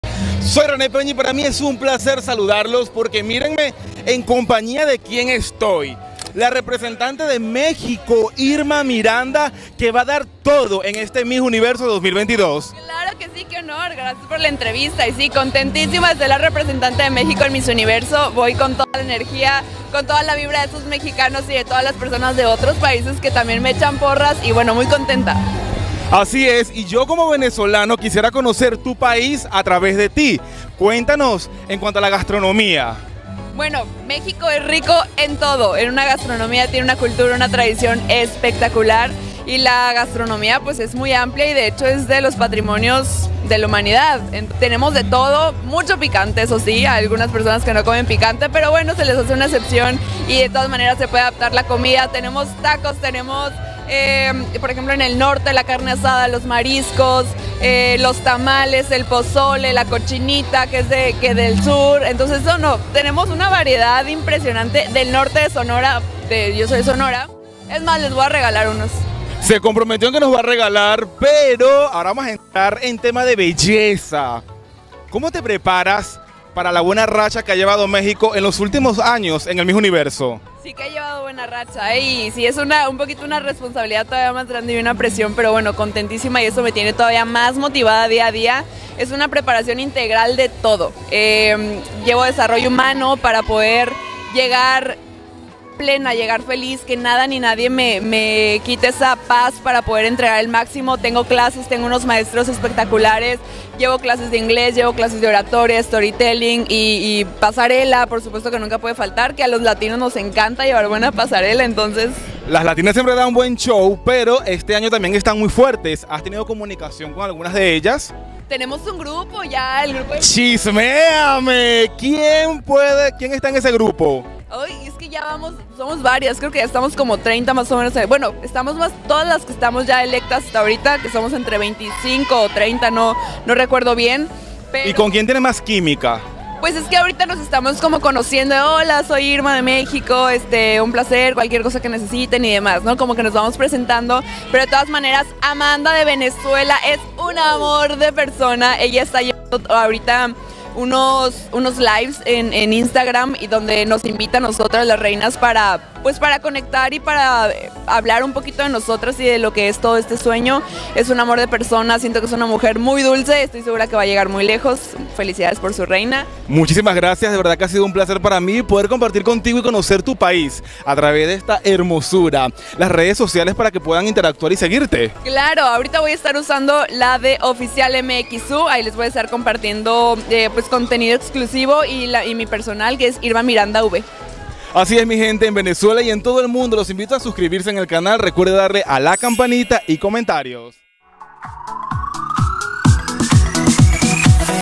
Irma Miranda Miss México 2022 ¿Qué hará para GANAR el Miss Universo? | De cerca con Renee
Miss México Irma Miranda se prepara para dar la batalla en la próxima edición del Miss Universo 2022.

Mis mejores deseos, es una chica con grandes cualidades y una energía impresionante que la llevará muy lejos en la industria del entretenimiento.

¿Crees que ganará la corona del concurso?